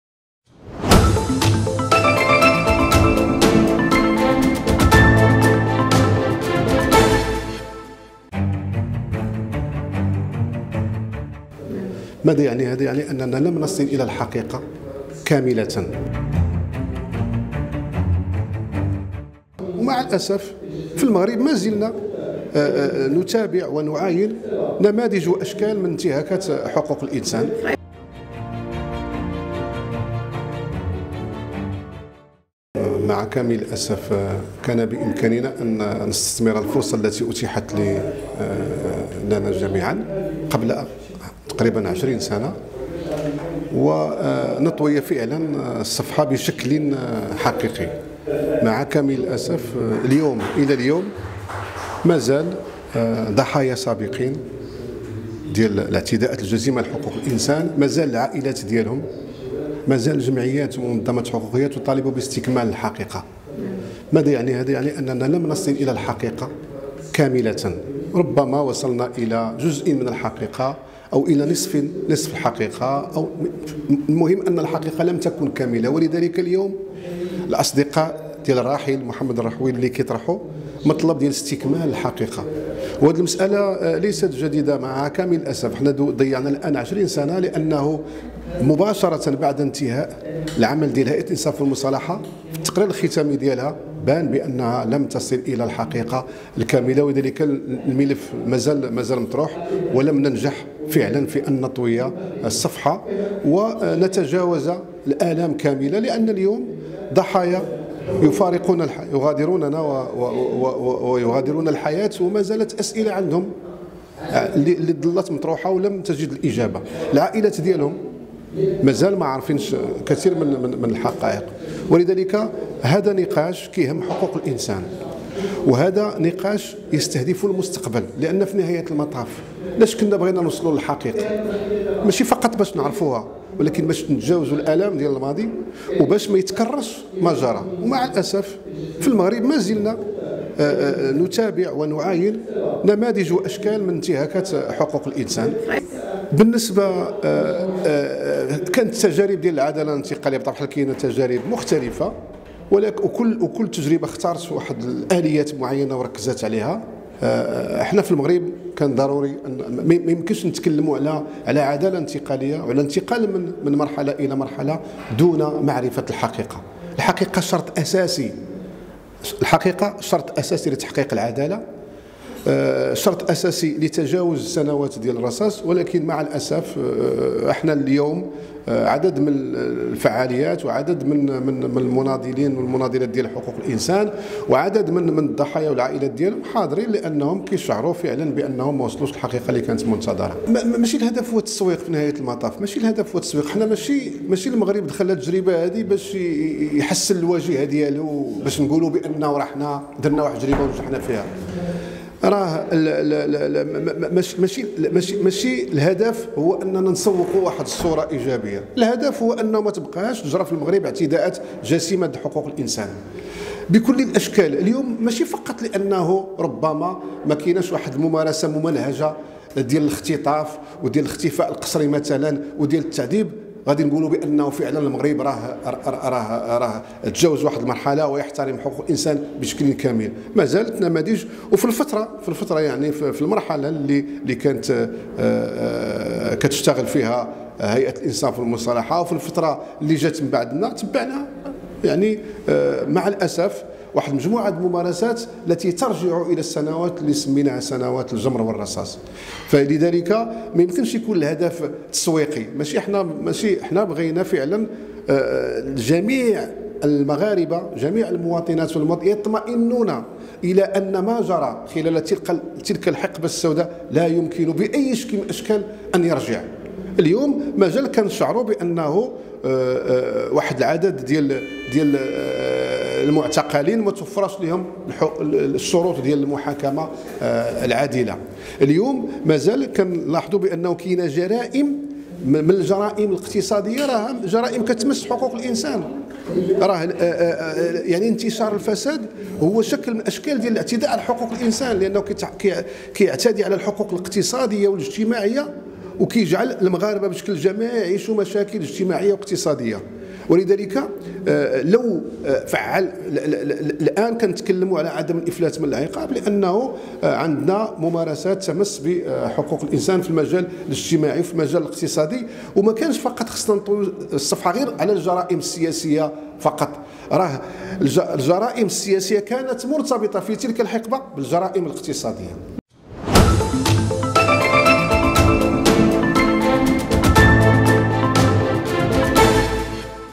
محمد حفيظ: رغم مرور 20 سنة لازالت الحقيقة غائبة في ملف " سنوات الرصاص"
في ندوة فكرية تحت شعار من "أجل حقيقة كاملة وذاكرة متقدة"، بمناسبة حفل تأبين الفقيد محمد الرحوي، أحد المعتقلين السياسيين، في ملف "مجموعة بنو هاشم"، محمد حفيظ، أستاذ باحث بجامعة الحسن الثاني بالدار البيضاء، الصحافي السابق، يؤكد على ضرورة إجلاء الحقيقة فيما بعرف بملف " سنوات الرصاص"، وذلك للطي النهائي لهذا الملف.